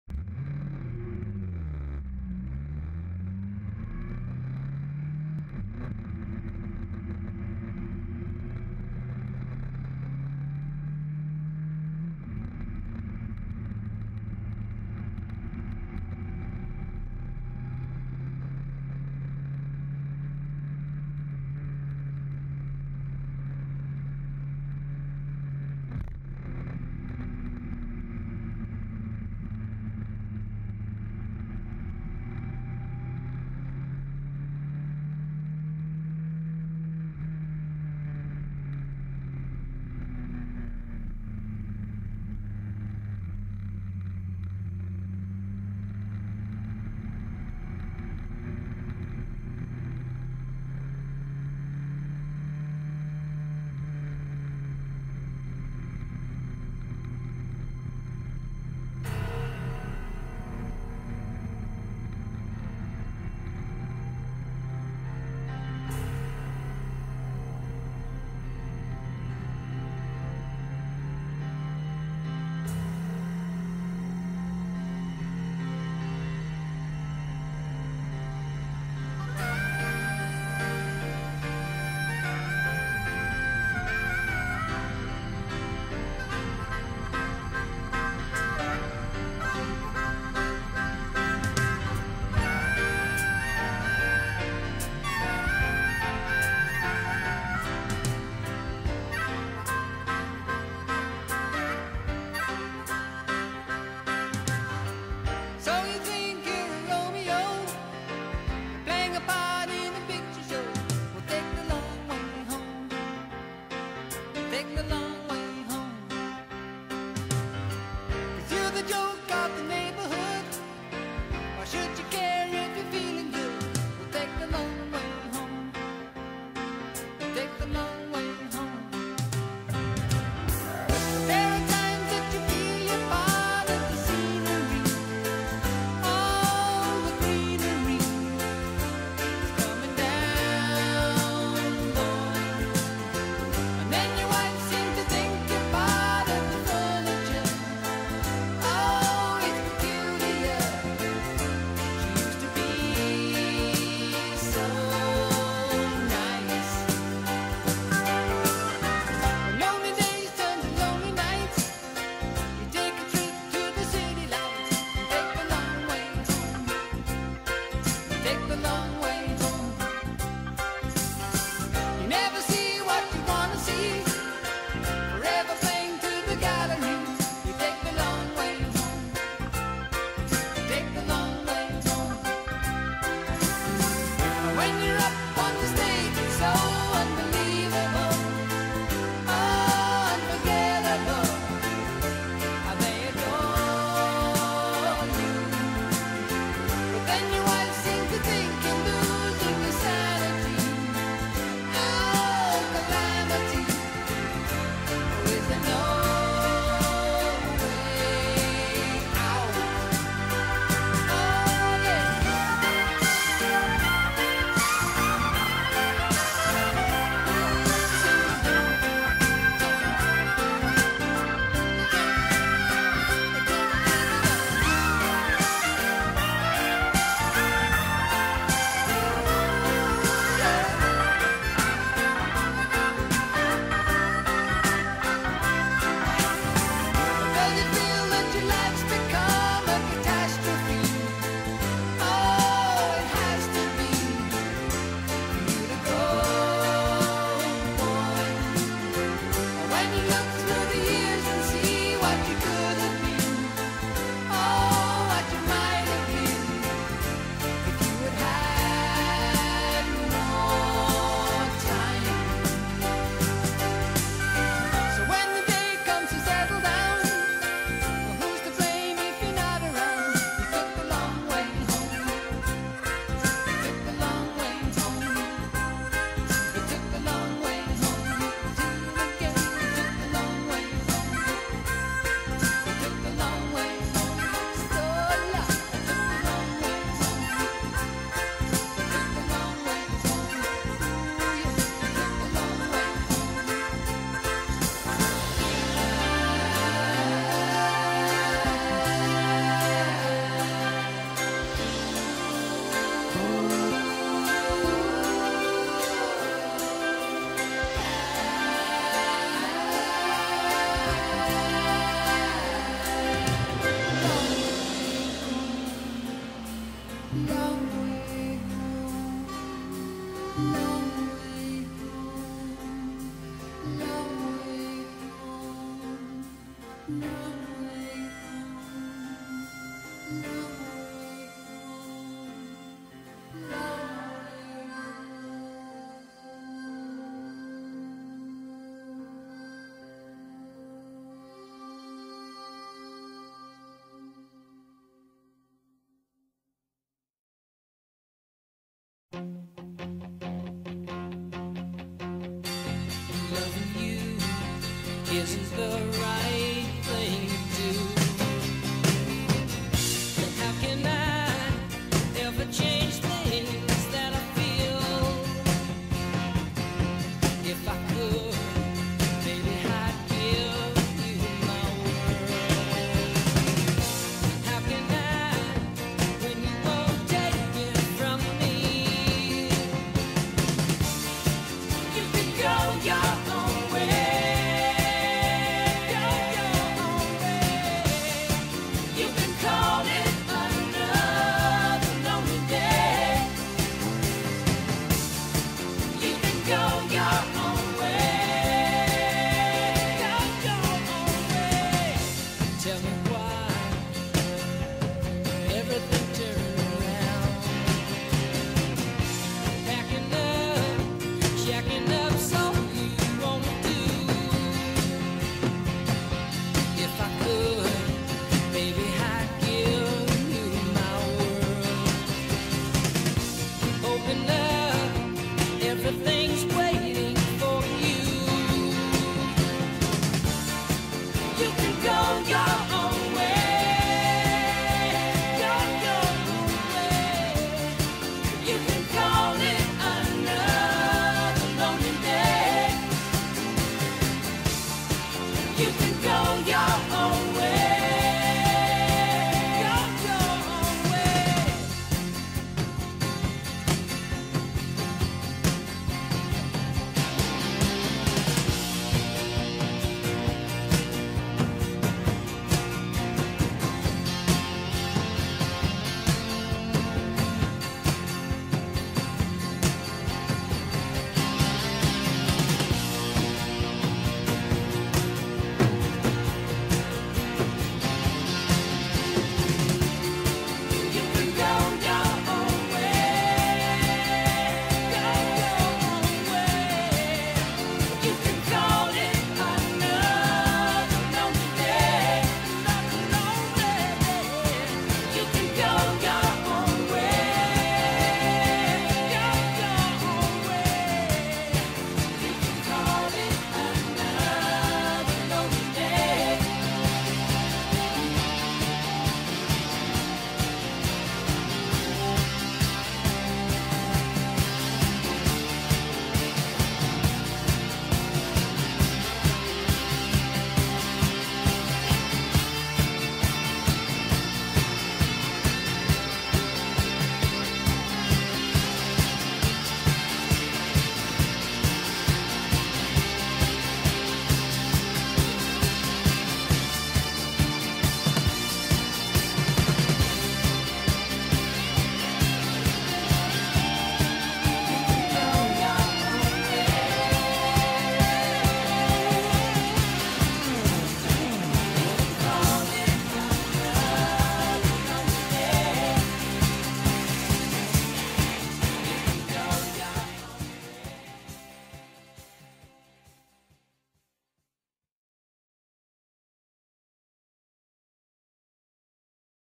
D1006 Lanslebourg Mont Cenis
Ascent to Lac du Mont Cenis on my GTR 1400